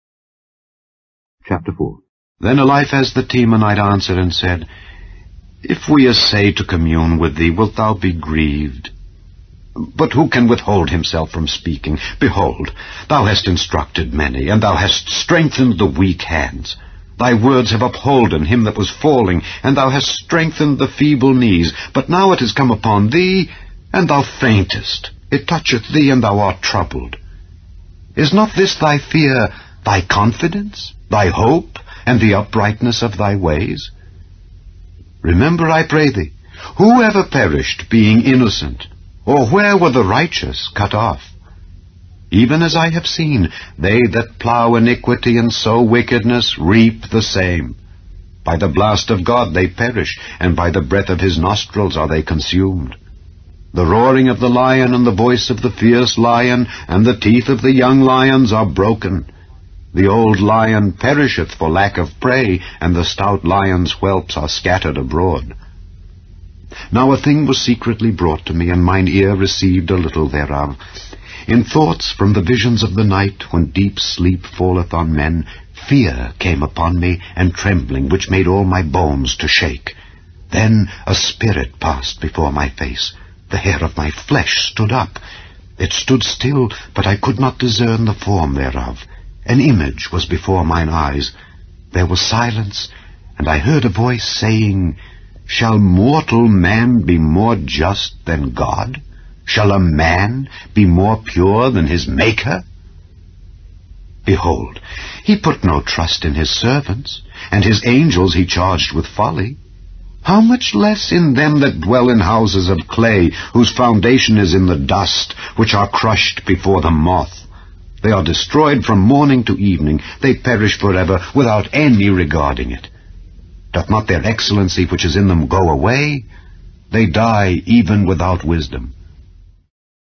Chapter-By-Chapter Bible Reading - Job 4
Job is the 18th book of the Bible and is part of the Old Testament.

Isaiah 55:11: